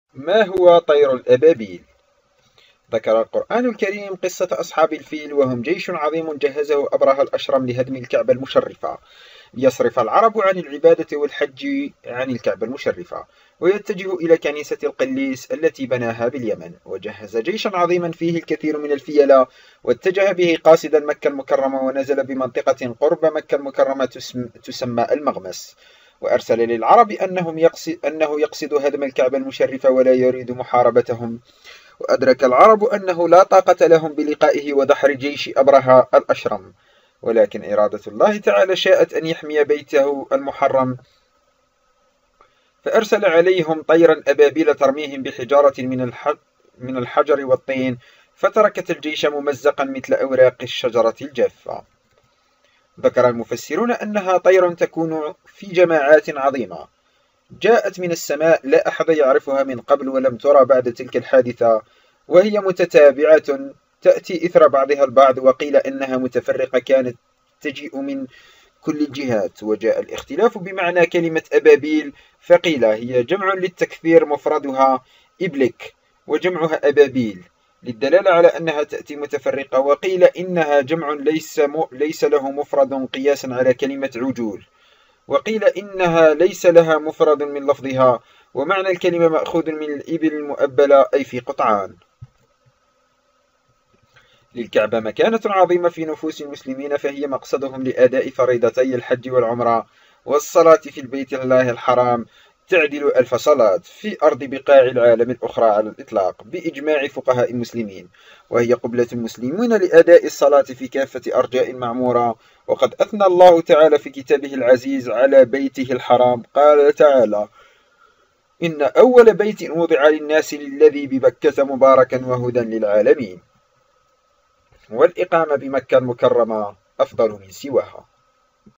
ما هو طير الابابيل ؟
ذكر القرآن الكريم قصة أصحاب الفيل، وهم جيش عظيم جهّزه أبرهة الأشرم لهدم الكعبة المشرفة؛ ليصرف العرب عن العبادة والحجّ عن العكبة المشرفة، ويتجهوا إلى كنيسة القليس التي بناها باليمن، وجهّز جيشاً عظيماً فيه الكثير من الفيلة، واتجه به قاصداً مكة المكرمة ونزل بمنطقة قرب مكة المكرمة تسمى المغمس، وأرسل للعرب أنّه يقصد هدم الكعبة المشرفة ولا يريد محاربتهم، وأدرك العرب أنّه لا طاقة لهم بلقائه ودحر جيش أبرهة الأشرم، ولكن إرادة الله تعالى شاءت أن يحمي بيته المحرم، فأرسل عليهم طيراً أبابيل ترميهم بحجارة من الحجر والطين، فتركت الجيش ممزقاً مثل أوراق الشجرة الجافة.[١] Volume 0%